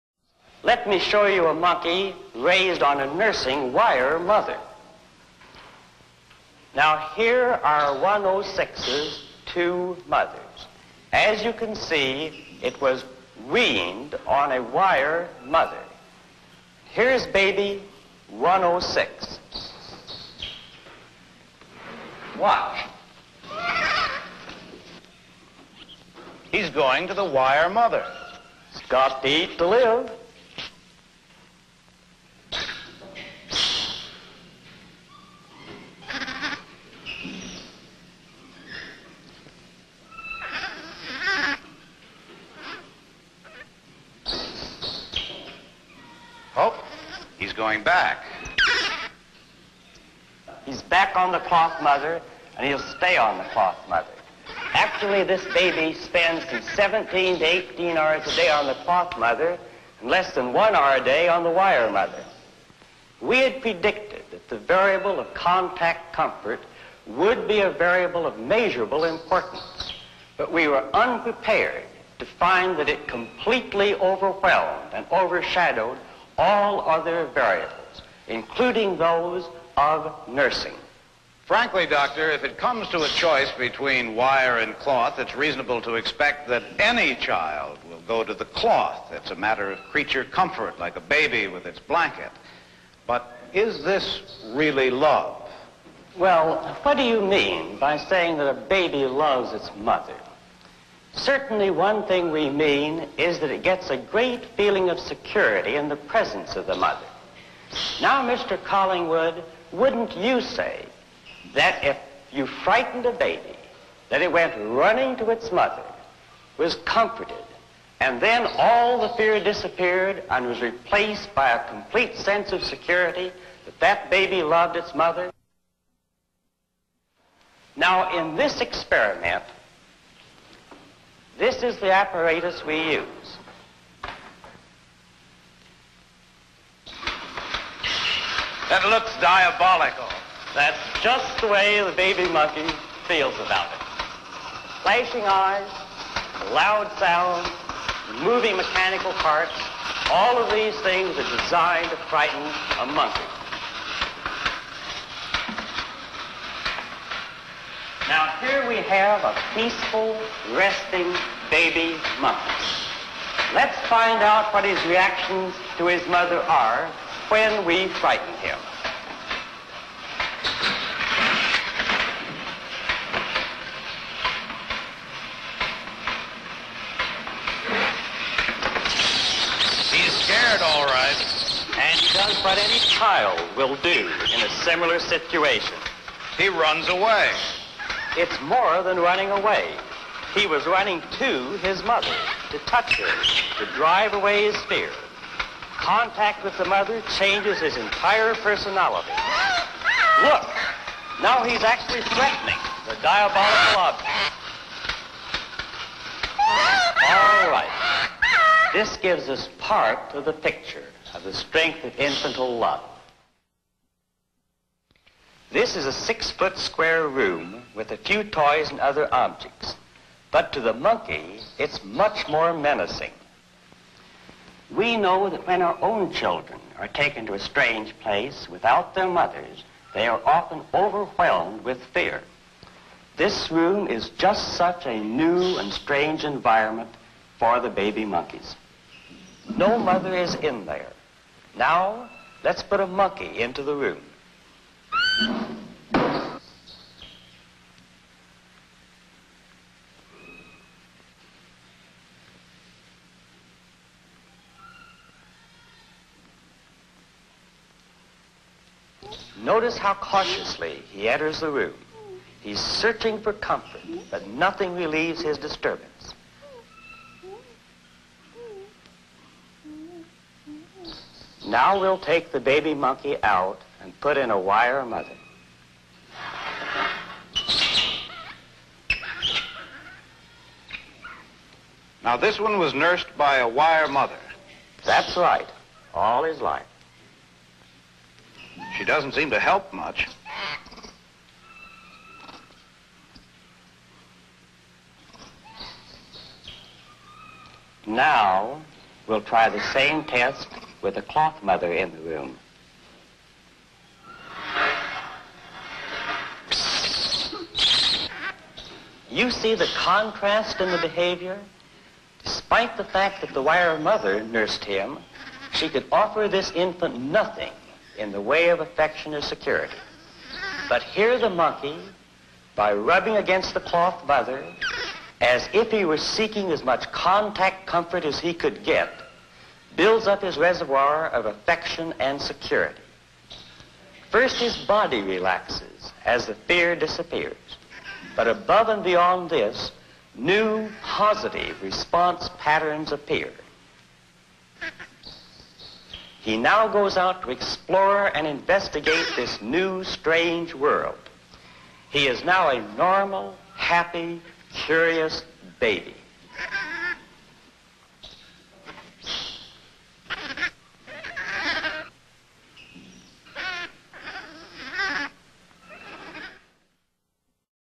Estudo de Harlow sobre dependência em macacos LEGENDADO
Estudo de um cientista sobre as necessidades de um bebê macaco em relação à sua mãe. Através dos estudos, Harlow pode descobrir que o bebê se importa mais com a variável do conforto (no qual ele corre para a mãe que lhe conforta e lhe livra do medo) do que com a variável do alimento, da amamentação